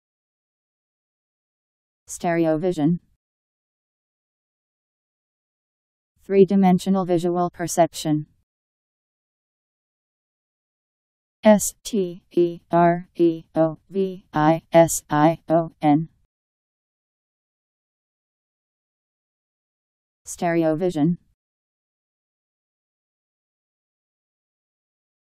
Stereovision Meaning
Video shows what stereovision means. three-dimensional visual perception.  Stereovision Meaning. How to pronounce, definition audio dictionary. How to say stereovision. Powered by MaryTTS, Wiktionary